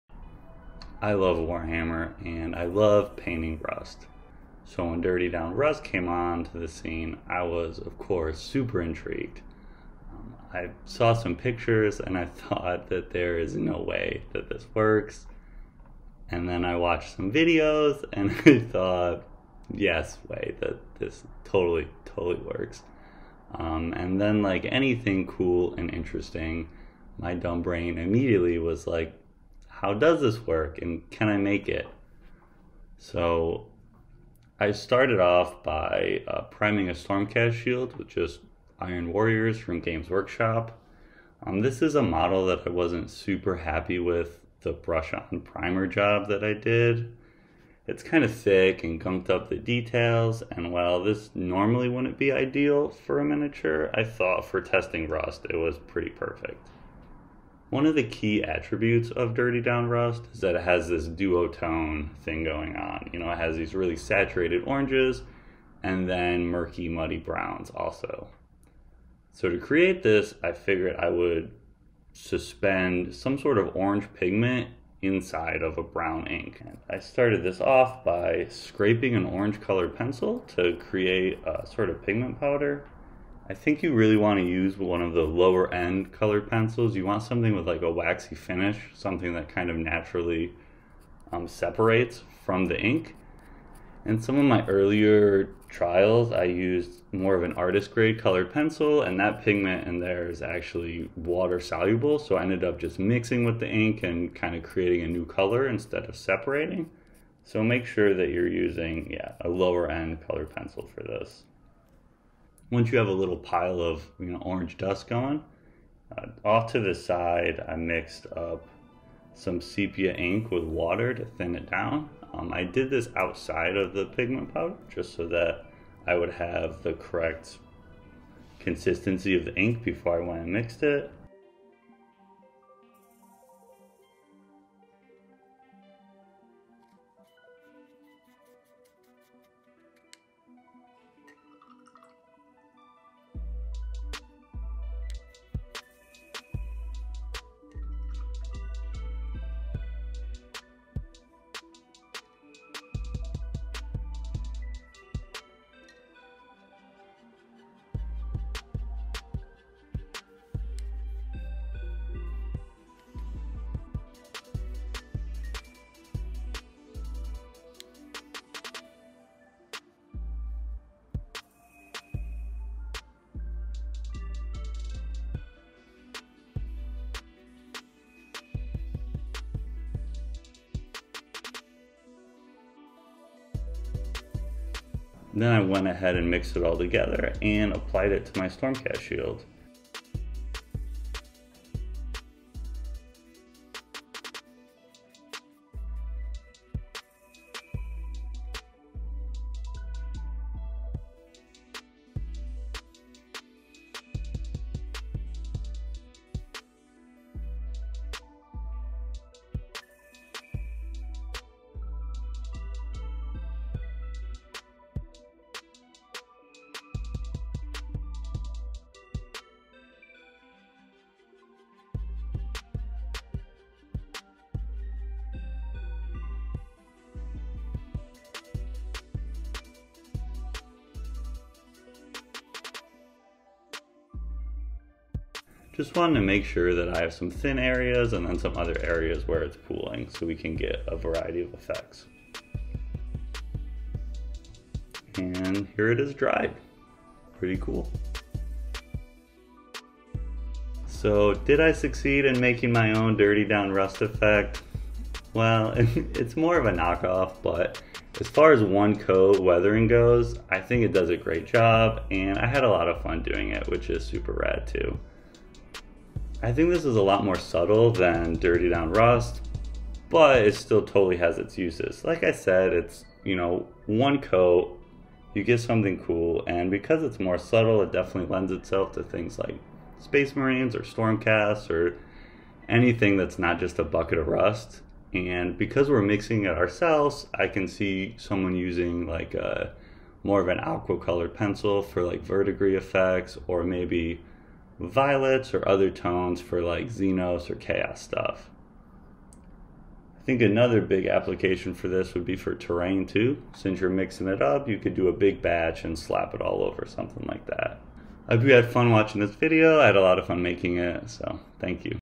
Can I recreate Dirty Down Rust Effect? | DIY Rust solution
Making my own DIY Dirty Down Rust weathering effect with some common items | Warhammer

Ink used in the video is Daler-Rowney FW Acrylic Ink

Any purchase made gives me a small amount of money at no cost to you